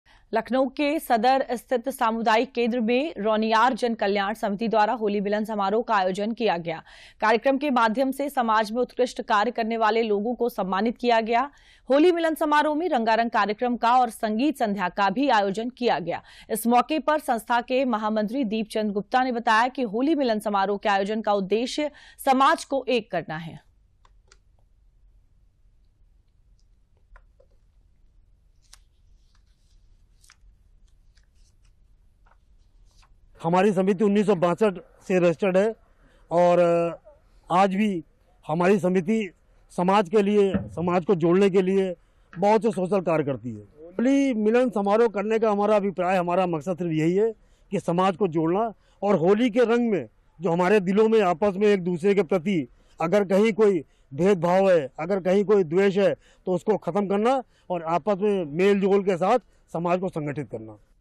Lucknow: रौनियार जन कल्याण समिति द्वारा होली मिलन समारोह का किया गया आयोजन
लखनऊ के सादर स्थित सामुदायिक केंद्र में रौनियार  जन कल्याण समिति द्वारा होली मिलन समारोह का आयोजन किया गया। कार्यक्रम के माध्यम से समाज मे उत्कृष्ठ कार्य करने वाले लोगो सम्मानित किया गया। होली मिलन समारोह मै रंगारंग कार्यक्रम का और संगीत संध्या का भी आयोजन किया गया। इस मौके पर संस्था के महामंत्री दीप चंद गुप्ता ने बताया कि होली मिलन समरोह के आयोजन का उद्देश्य समाज को एक करना है।